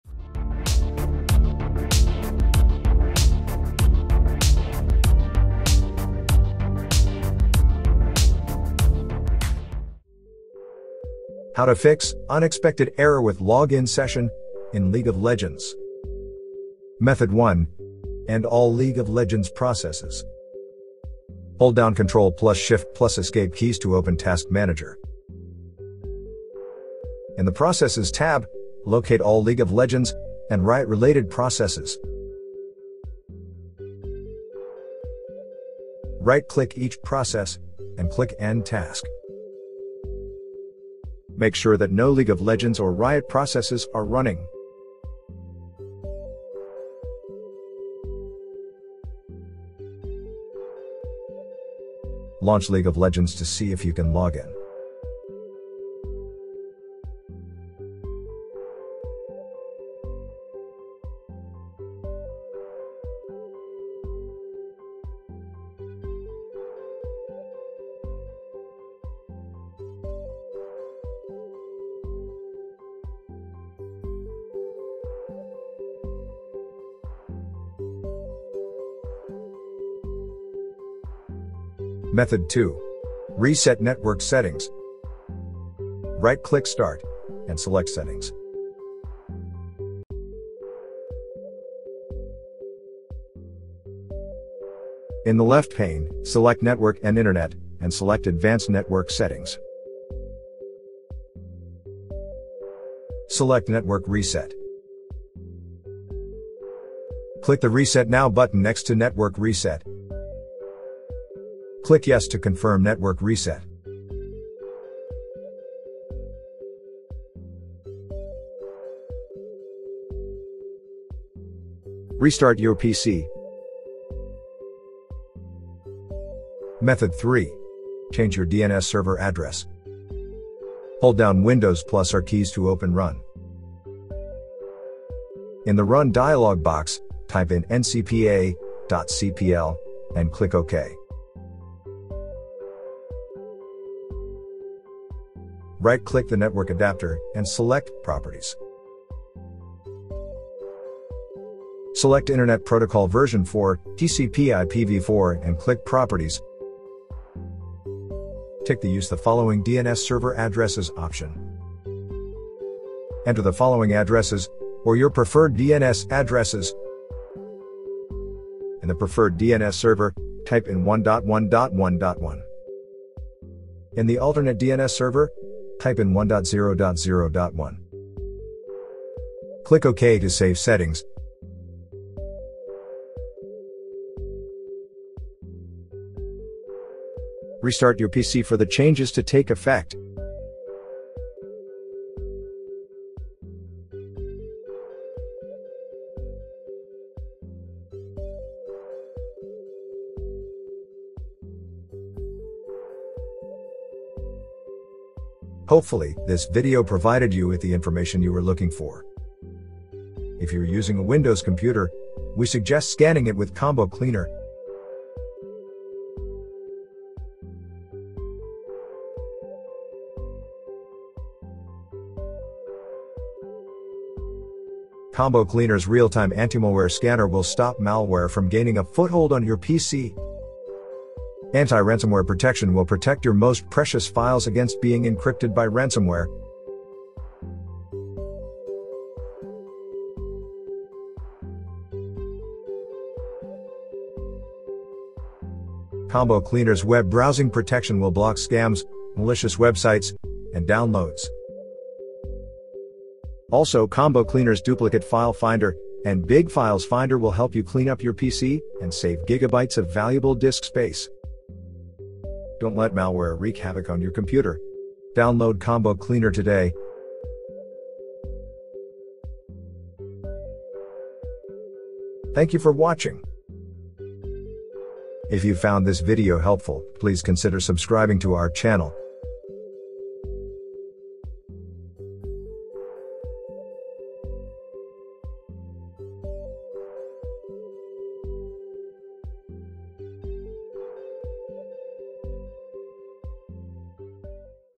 How to Fix "Unexpected Error With Login Session" in League of Legends?
Video showing how to Fix "Unexpected Error With Login Session" in League of Legends.